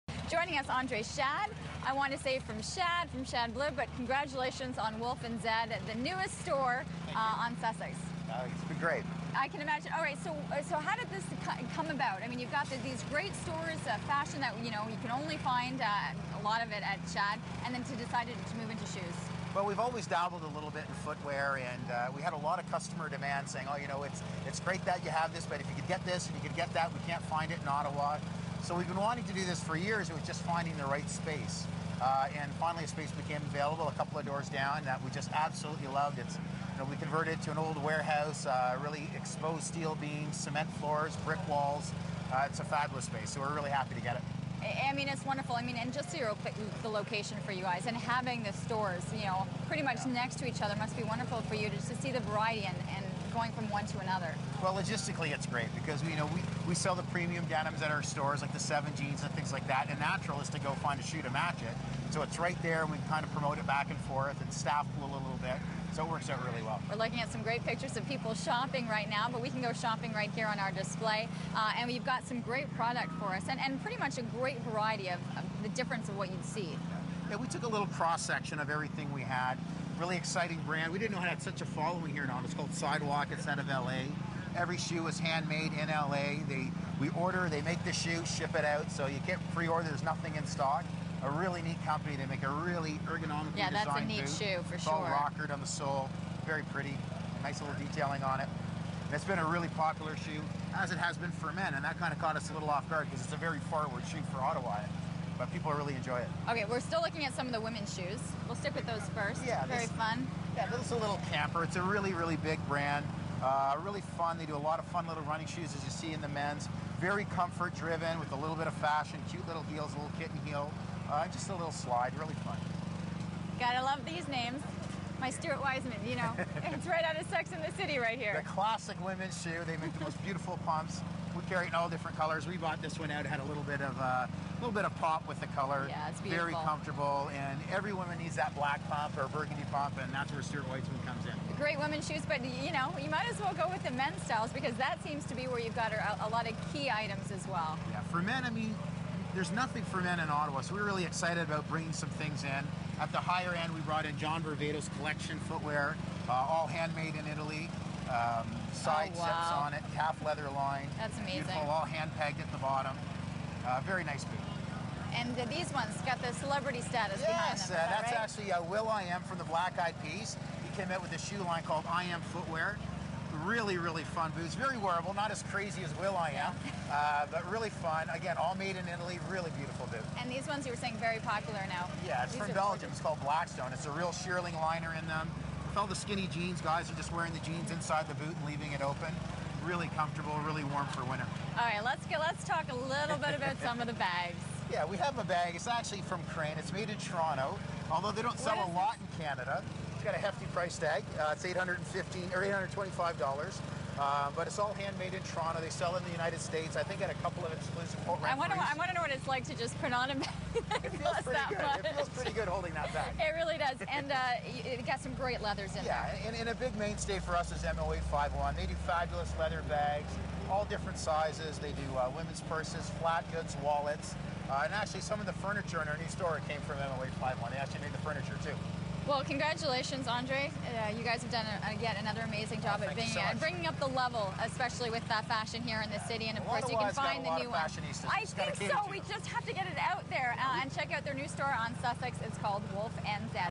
Wolfe and Zed
Shoes from a new store in the ByWard Market, Wolfe and Zed.